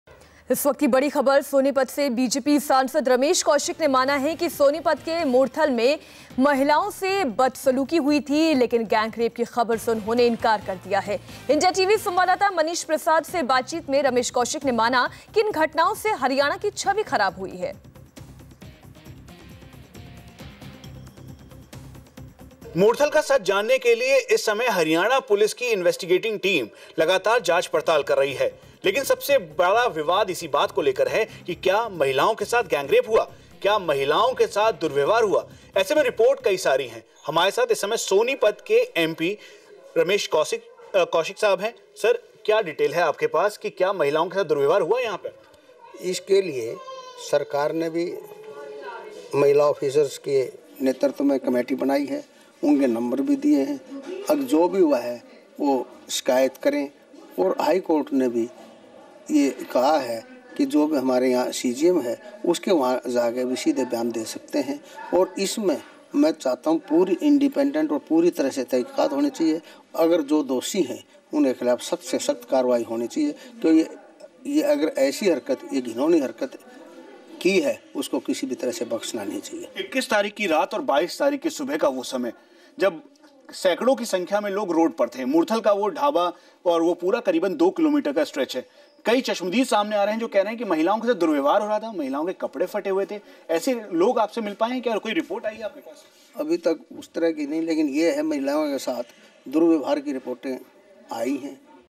BJP MLA Ramesh Kaushik on Murthal Gangrapes During Jat Reservation Stir
BJP MLA Ramesh Kaushik talks about Murthal gangrapes during Jat reservation stir.

According the reports dozens of women were gang raped during the violent protest by Jats for reservation in Murthal. Haryana police and government appealed the victims to come forward and tell the truth. A woman has come forward and told the Haryana Police that she was pulled out of a car and gang-raped. During the violent protest by Jats Haryana police caught running away from the spot of violence which triggered the anger among public.